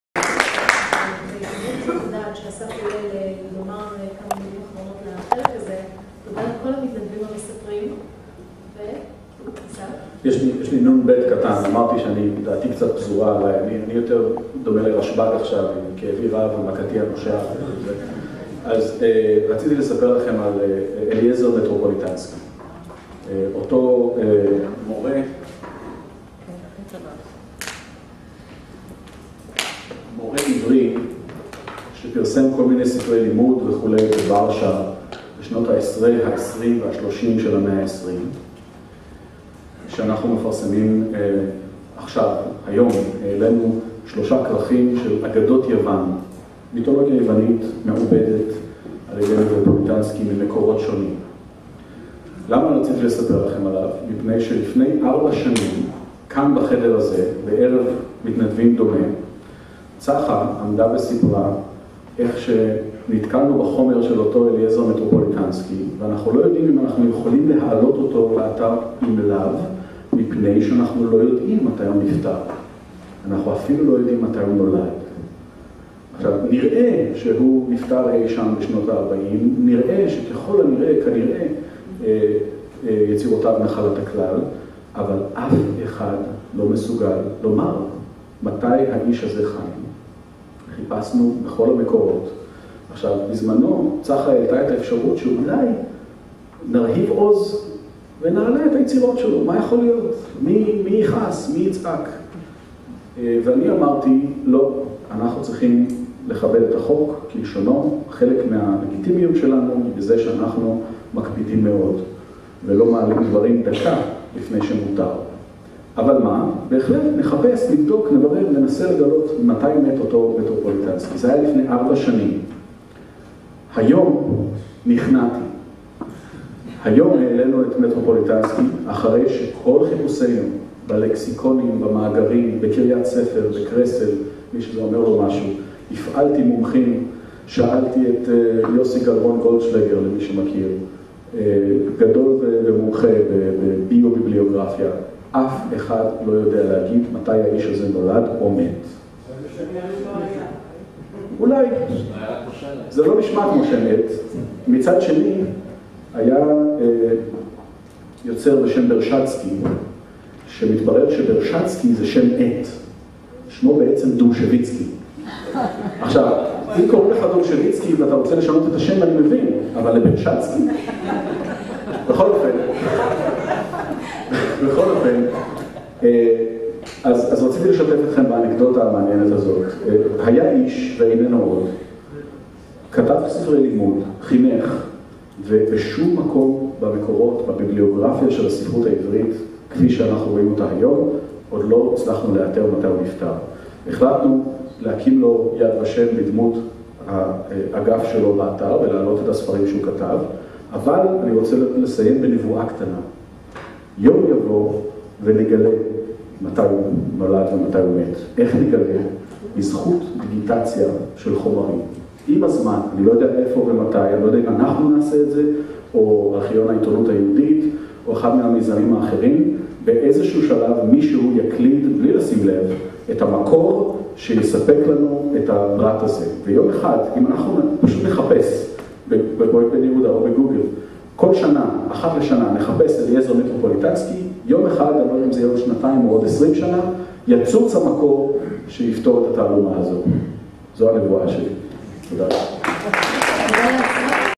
מתנדבי פרויקט בן-יהודה: אסף ברטוב - חלק ב
צולם בערב הוקרה למתנדבי פרויקט בן-יהודה בסימן שירת ימי הביניים, אשר התקיים ב- 15.09.16 באוני' ת"א.